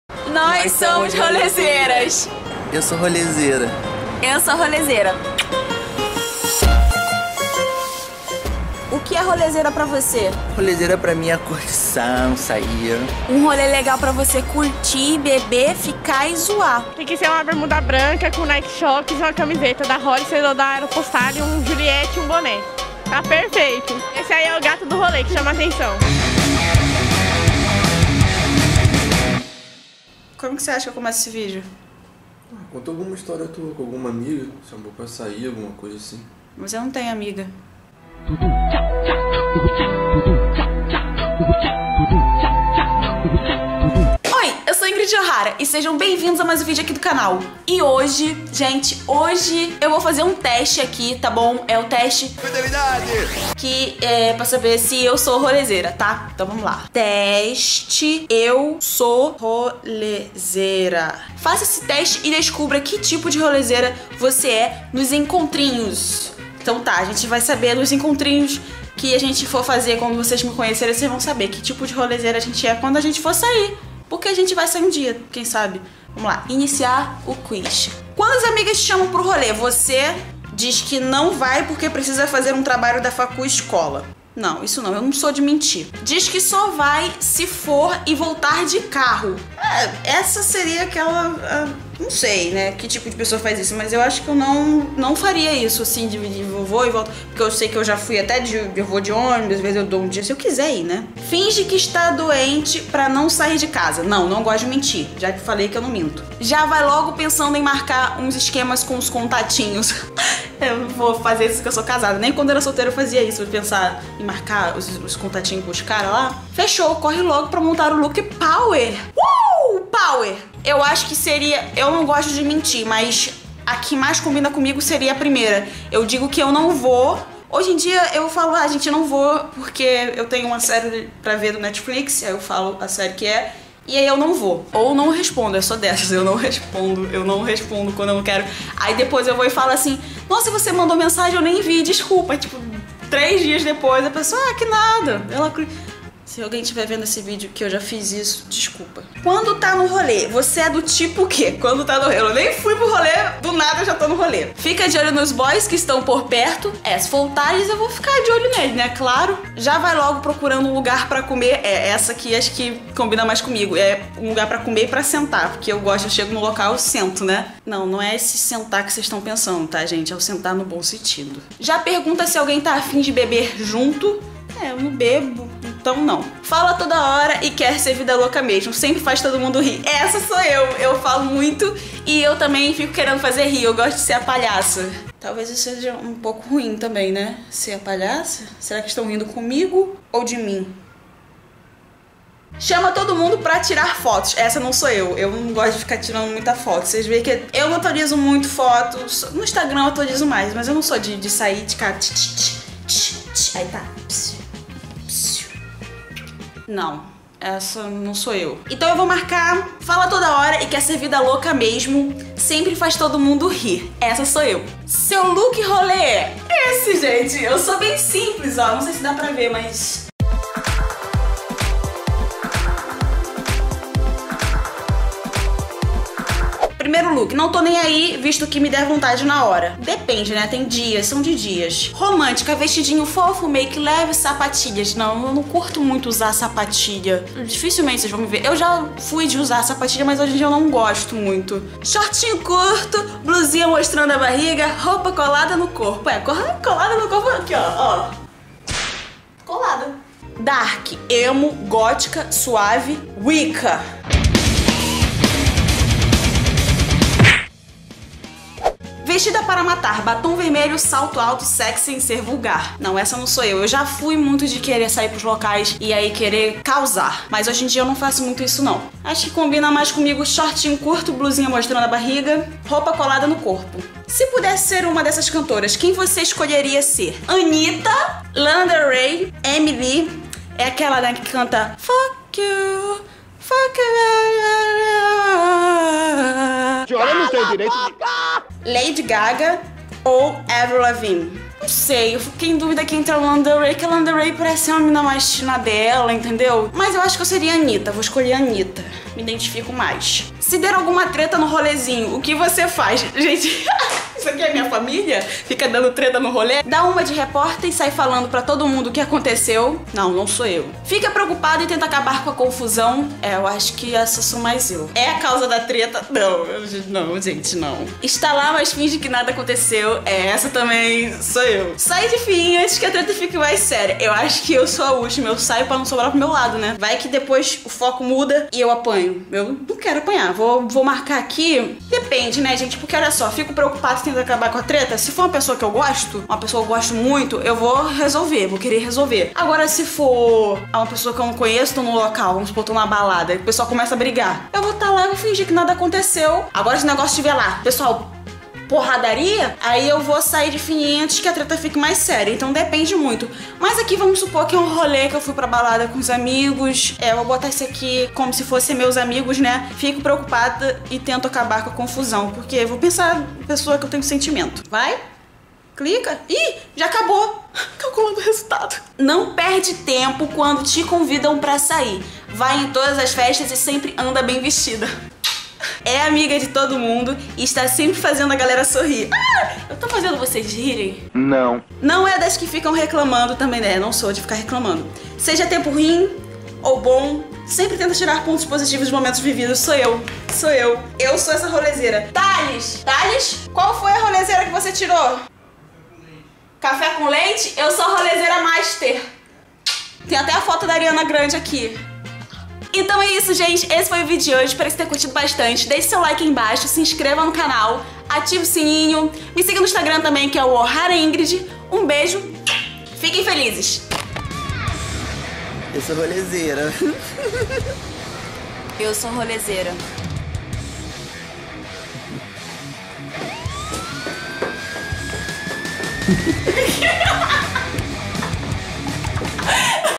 TESTE QUE TIPO DE ROLEZEIRA É VOCÊ!
Teste que tipo de rolezeira é você com apenas 5 perguntas. Beijos, beijos!

Snap: oharaingrid

Edição: Sgrenders